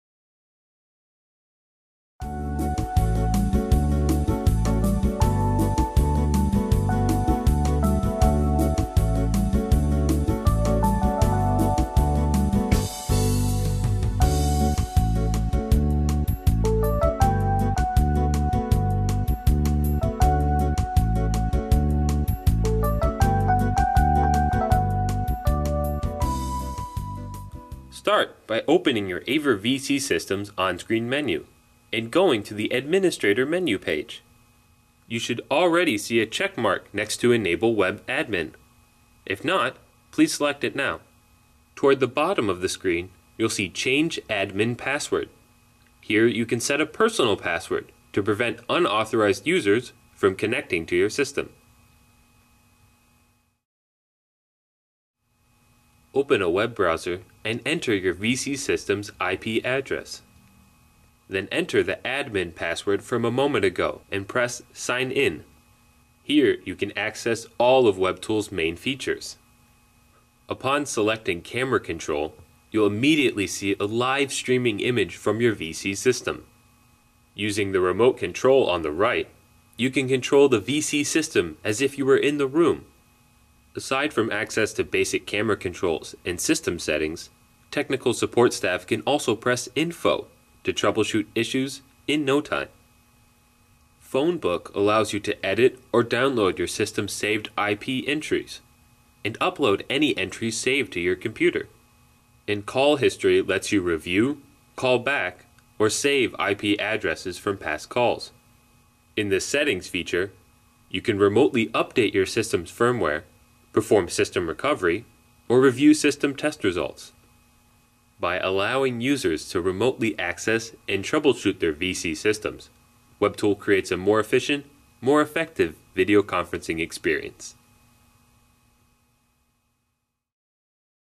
WebTool Introduction Video
Manage your AVer video conferencing system remotely via the Internet for fast and effective troubleshooting.